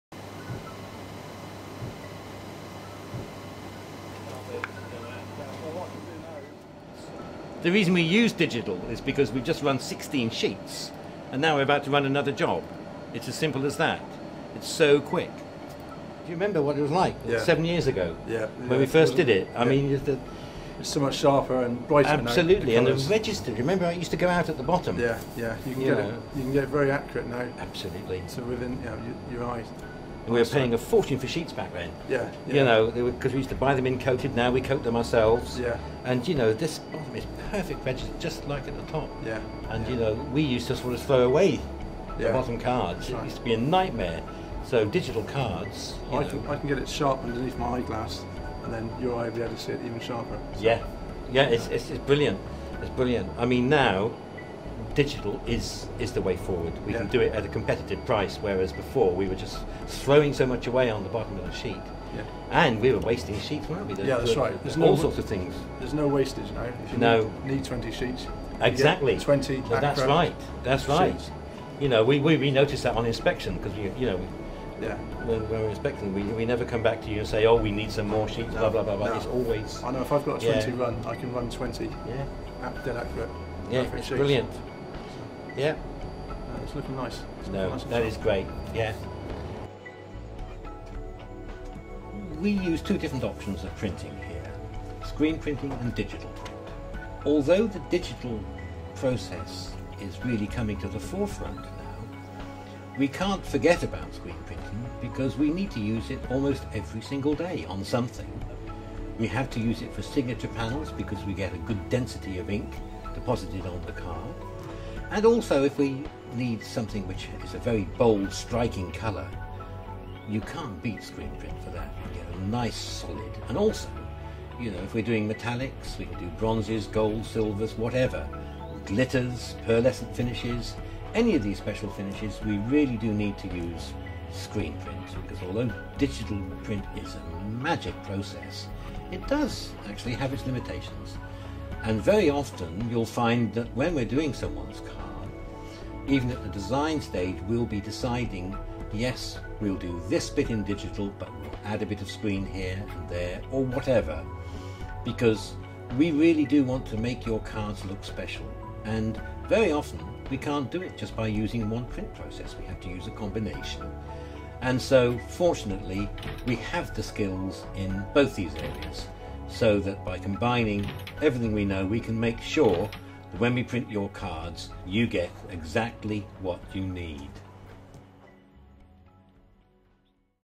Plastic Card Print Process
How the print process works when printing plastic cards at the Concept Card printing factory - membership, reward, loyalty, ID, hopsitality and business cards. Find out more on how we can help